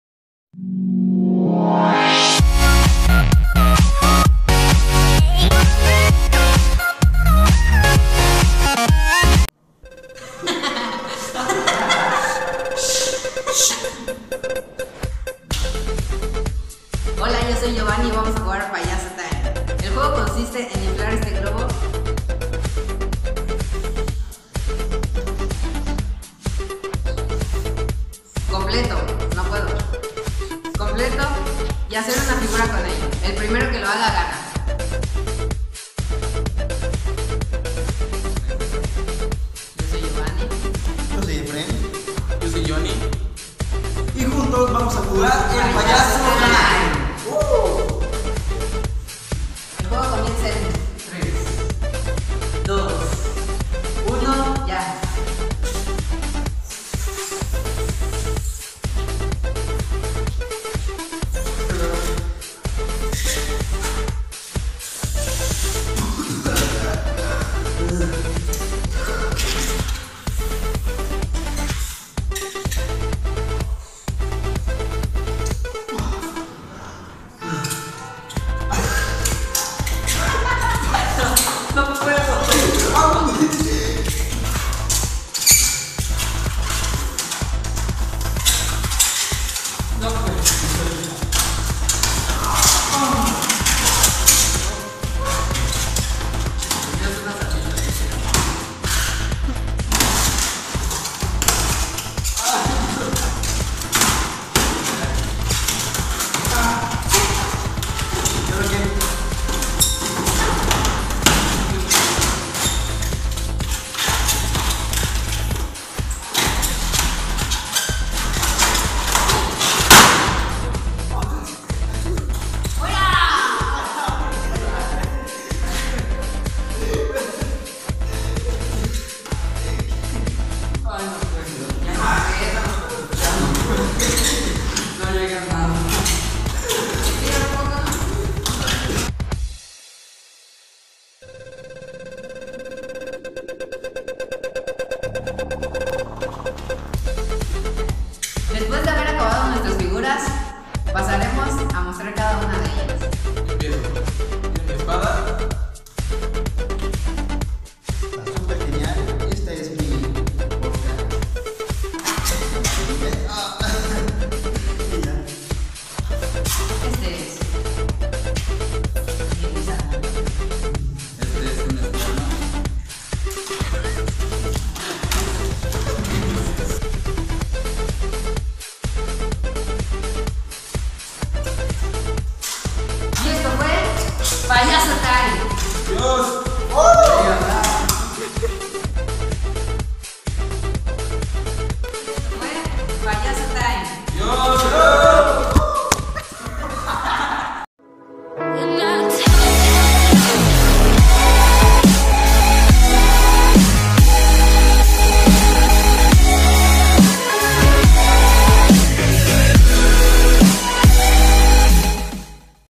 Payaso Time Challengue!! - Con Giovany, Ramses de paz y Cesar... 😂😂
Hola chicos como están, estamos de vuelta con un nuevo video y esta vez no traemos un Gameplay pero si traemos un reto, así que espero que realmente les guste y que les saque unas risas. 😝😝